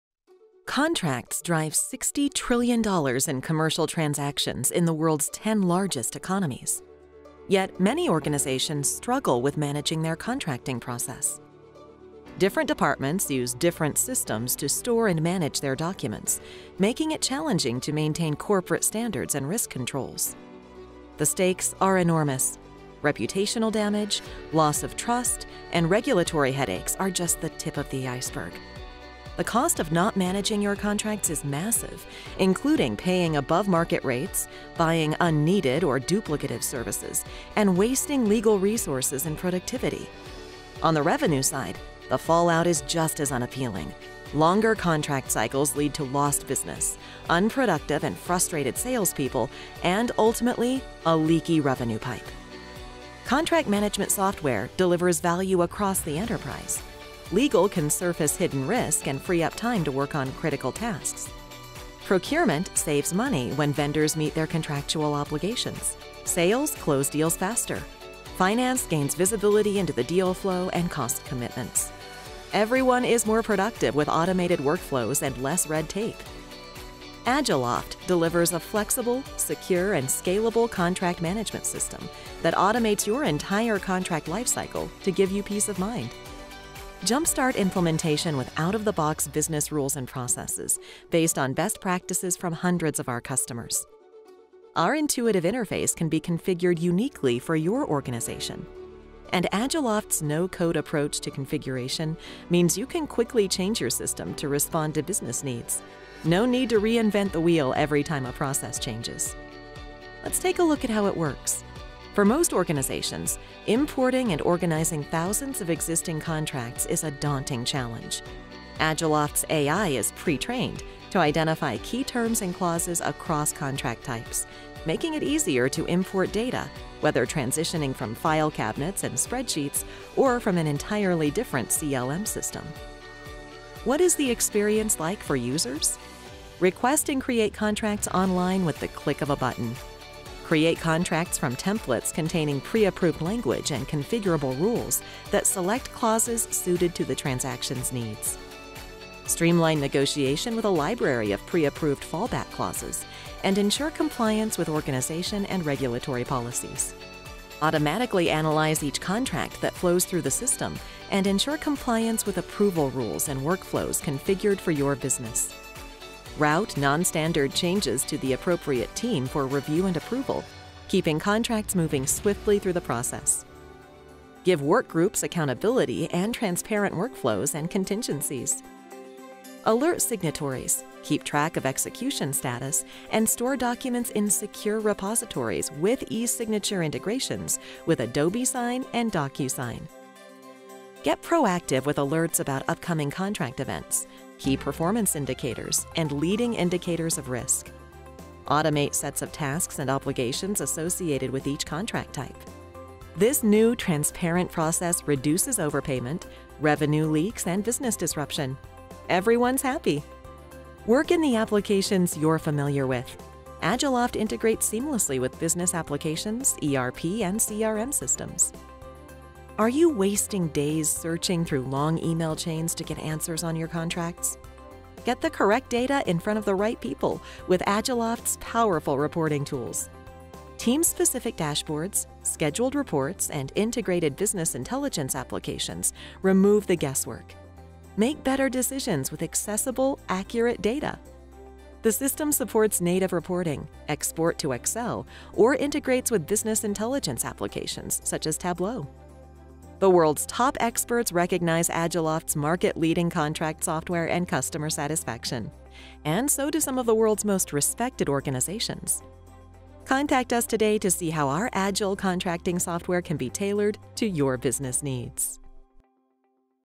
Agiloft Contract Lifecycle Management
A quick demo of Agiloft's Contract Lifecycle Management software.